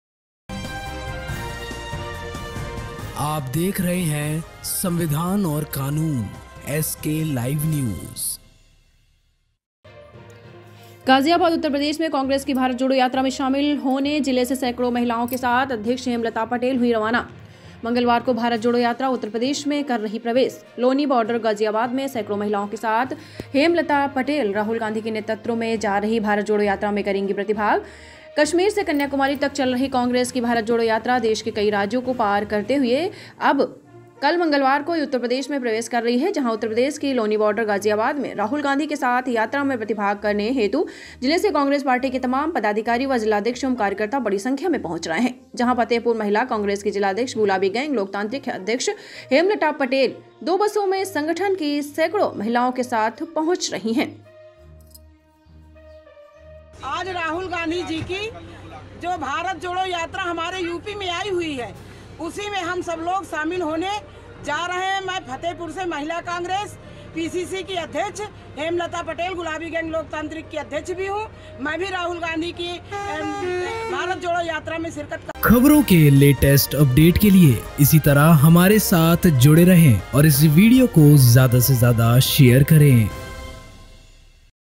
भारत जोड़ो यात्रा में शामिल हुई महिलाओं
गाजियाबाद यूपी में कांग्रेस की भारत जोड़ो यात्रा में शामिल होने जिले से सैकड़ों महिलाओं के साथ अध्यक्ष हेमलता पटेल हुईं रवाना

 मंगलवार को भारत जोडो यात्रा उत्तर प्रदेश में कर रही है प्रवेश

लोनी बार्डर गाजियाबाद में सैकड़ो महिलाओं के साथ हेमलता पटेल राहुल गाँधी के नेतृत्व में की जा रही भारत जोड़ो यात्रा में करेंगी प्रतिभाग

कश्मीर से कन्याकुमारी तक चल रही कांग्रेस की भारत जोड़ो यात्रा देश के कई राज्यों को पार करते हुए अब कल मंगलवार को यूपी में प्रवेश कर रही है जहाँ यूपी की लोनी बार्डर गाजियाबाद में राहुल गाँधी के साथ यात्रा में प्रतिभाग करने हेतु जिले से कांग्रेस पार्टी के तमाम पदाधिकारी व जिलाध्यक्ष एवं कार्यकर्ता बड़ी संख्या में पहुंच रहे हैं जहाँ फतेहपुर महिला कांग्रेस की जिलाध्यक्ष गुलाबी गैंग लोकतान्त्रिक अध्यक्ष हेमलता पटेल 2 बसों में संगठन की सैकड़ो महिलाओ के साथ पहुंच रही हैं |